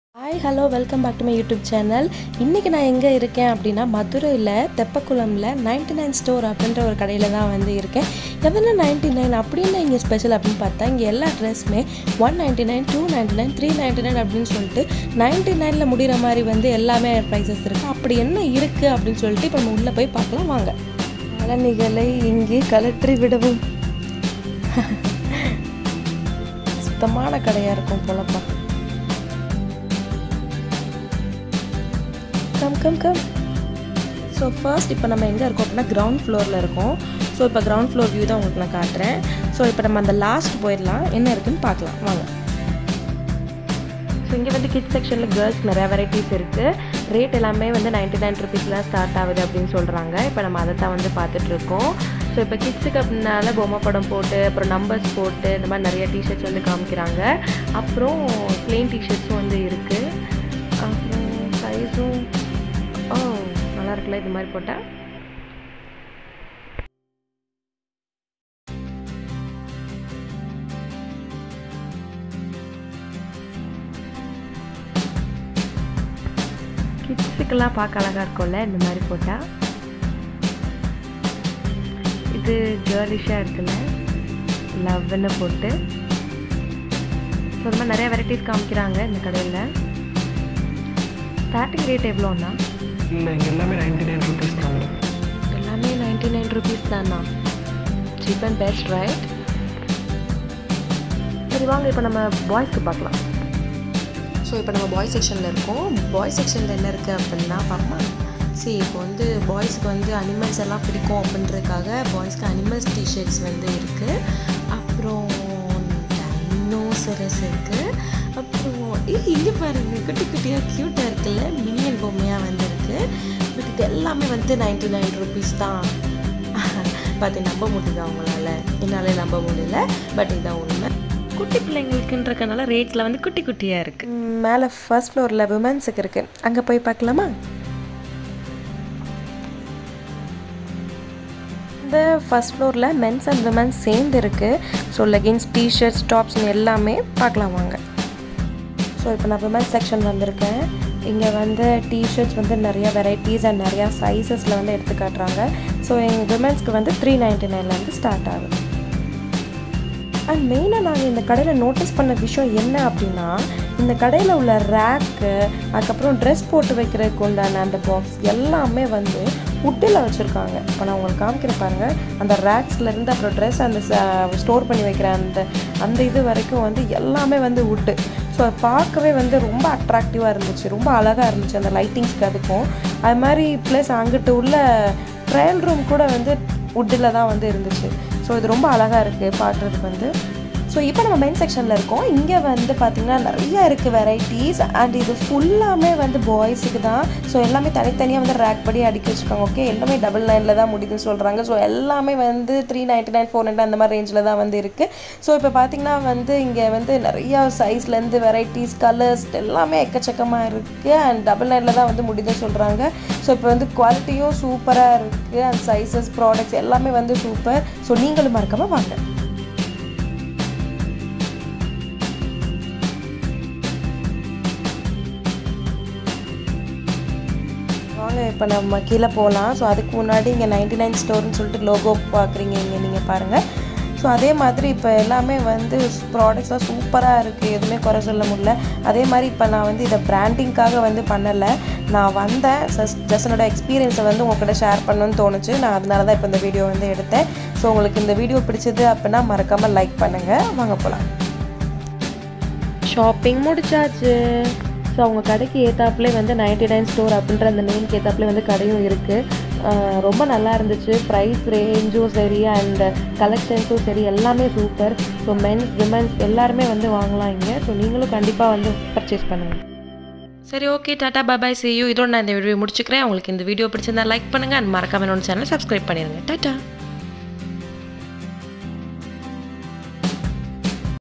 99store at madurai-starting price rs.99 only-99 store vlog moment-casual wears
99store, Kamarajar Salai, Madurai, Tamil Nadu

This was the address of 99 store..shop with your family This video is for best shopping at cheapest price with best quality# casual wears for kids,men and women#best shopping in madurai#lot of collections for kids